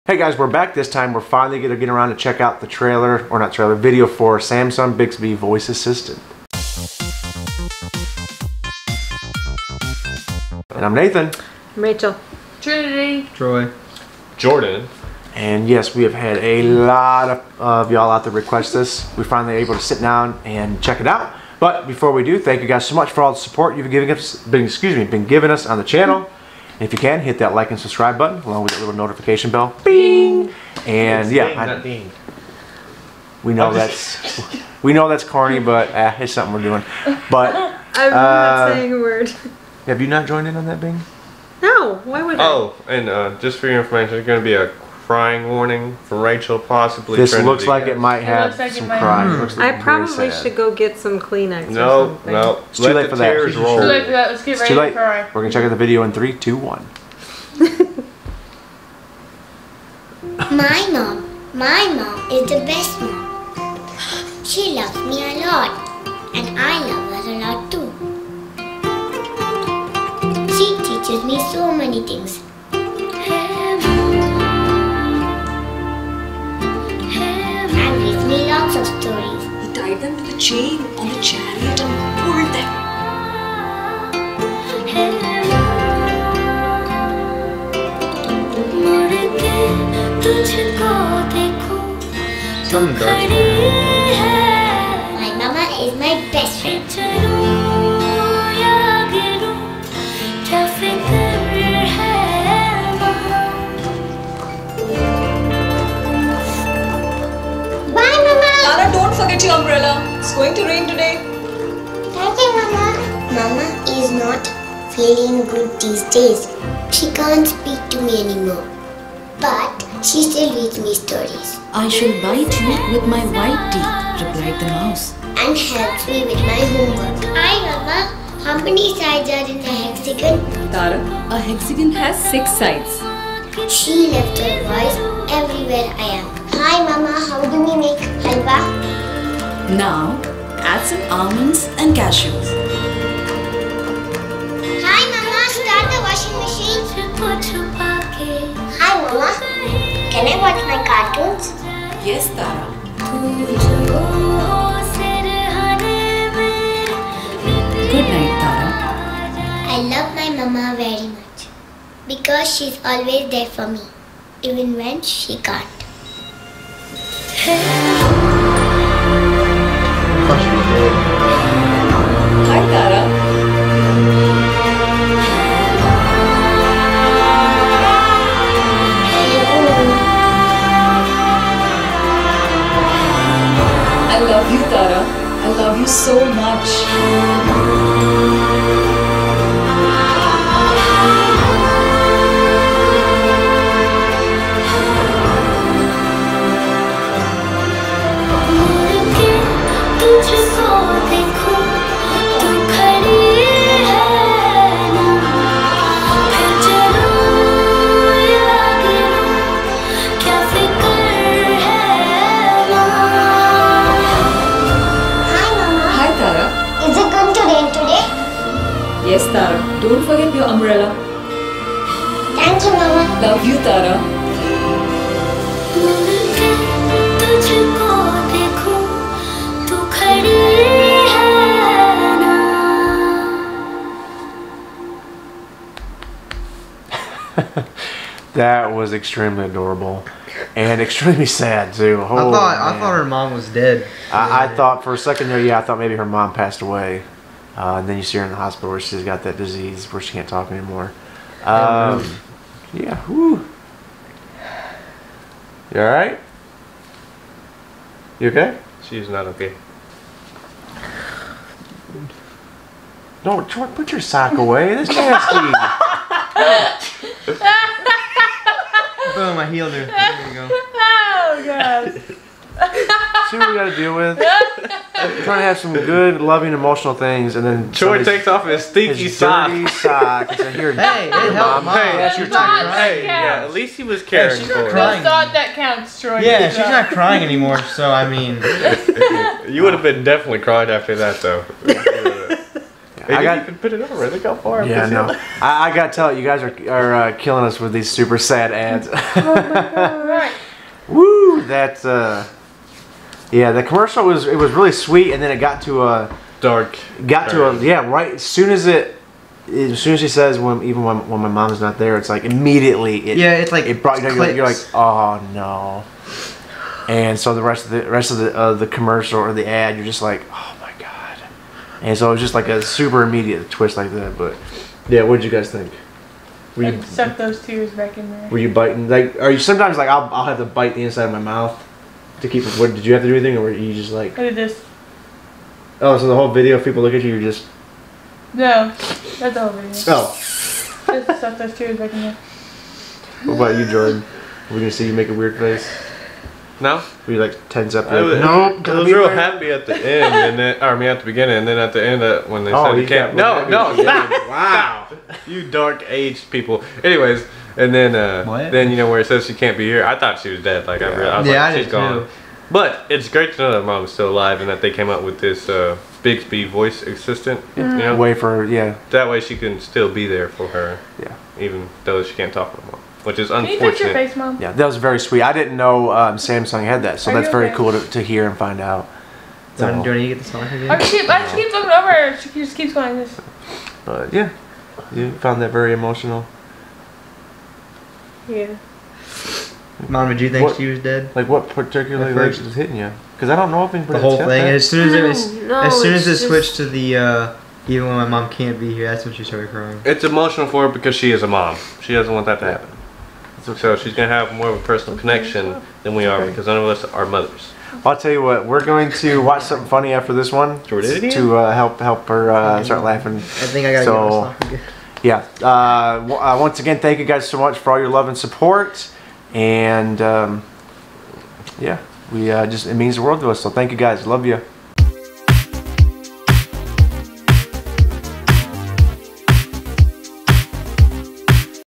Samsung Bixby Voice Assistant | REACTION! | Emotional Video!
We check out another very emotional ad video. This time it's for Samsung's Bixby Voice Assistant!